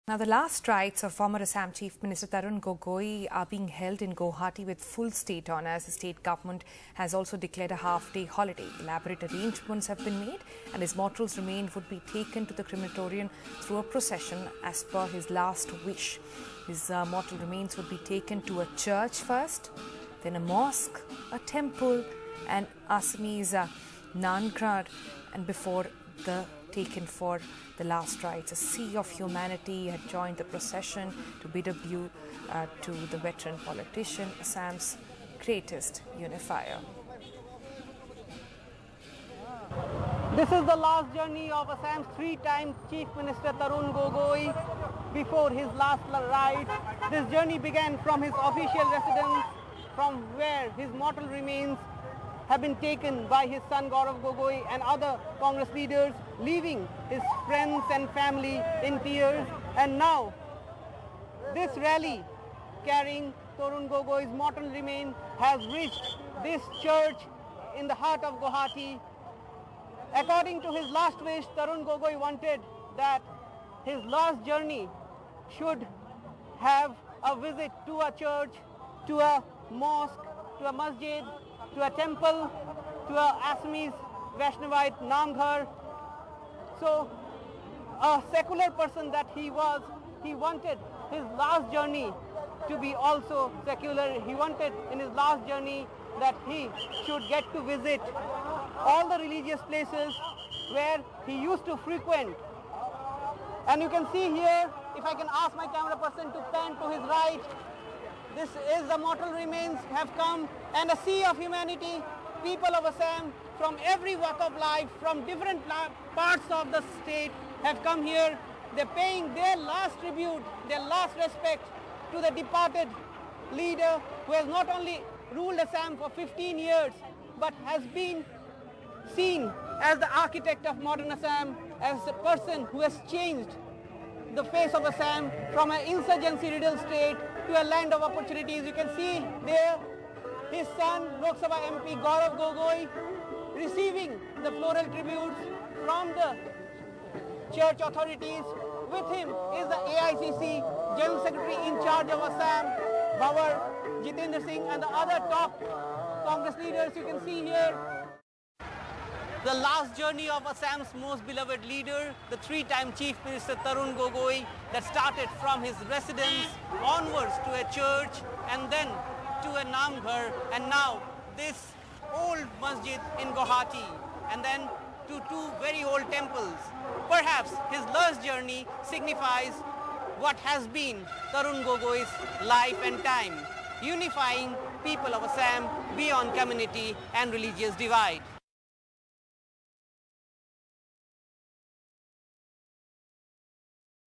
Thousands Pay Last Respects To Ex-Assam Chief Minister Tarun Gogoi
Former Assam chief minister Tarun Gogoi were paid last respects today by thousands in Guwahati where he will be cremated with full state honours. The Congress veteran died on Monday.